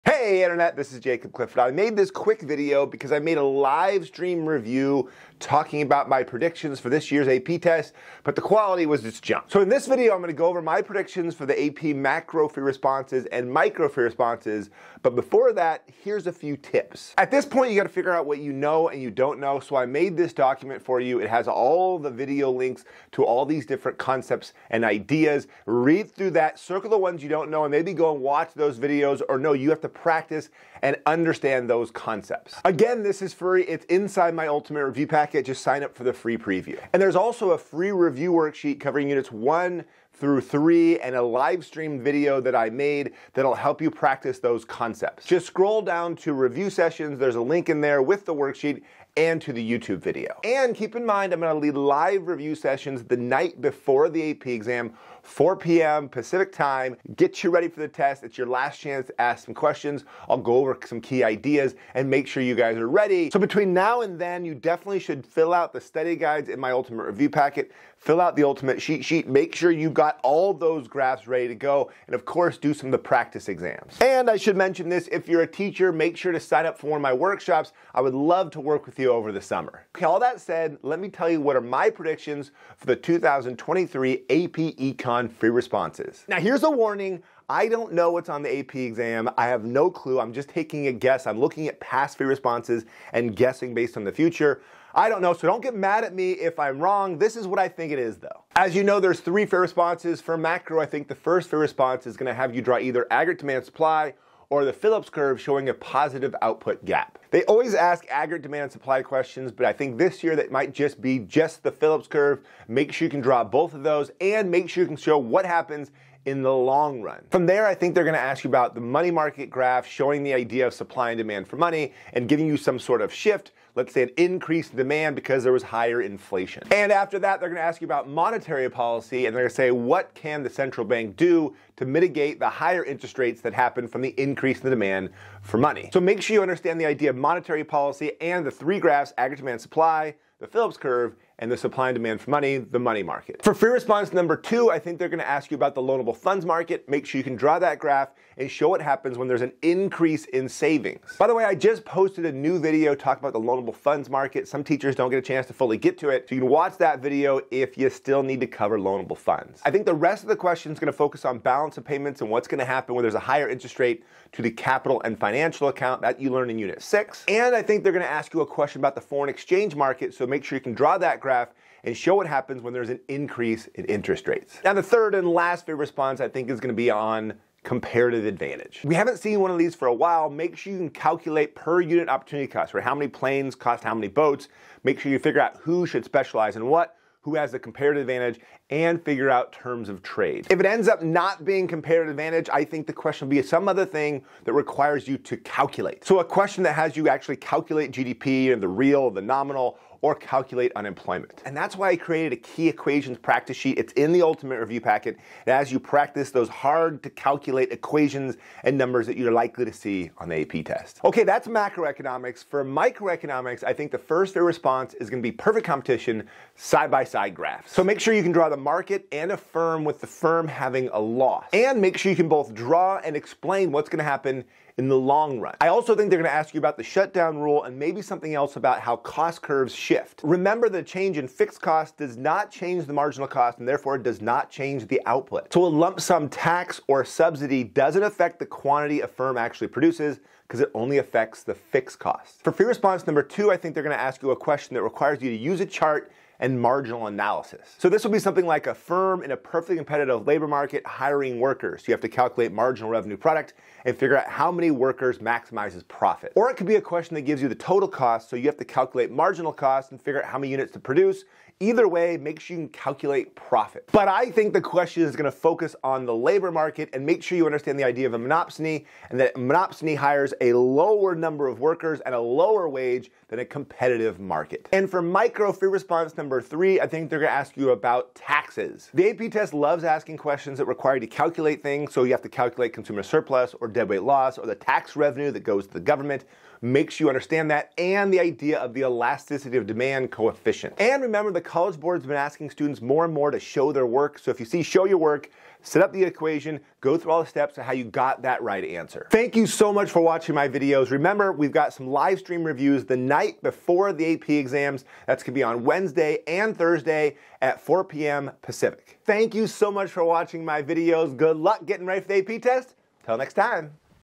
2023 FRQ Predictions (Updated)
My 2023 AP Macro and AP Micro exam FRQs predictions. For more help and practice, check out the Ultimate Review Packet

AP Macro
Macro FRQ #1: Phillips curve with positive output gap, long run adjustment, money market with shift, monetary policy with limited or ample reserves

Macro FRQ #2: Loanable funds with an increase in savings, affect on capital and financial account, foreign exchange and a change in interest rates

Macro FRQ #3: Comparative advantage, calculate the opportunity cost, terms of trade, connection to the PPC

AP Micro
Micro FRQ #1: Perfect Competition- Side-by-Side Graphs with firm making a loss (label area of loss), shut down rule, long-run adjustment,

Micro FRQ #2: Hiring workers in a perfectly competitive labor market, calculate the marginal revenue product, hire workers to maximize profit, calculate profit, compared to monopsony.

Micro FRQ #3: Supply and demand with a tax. Calculate consumer surplus, tax per unit or total tax revenue, deadweight loss and elasticity of demand coefficient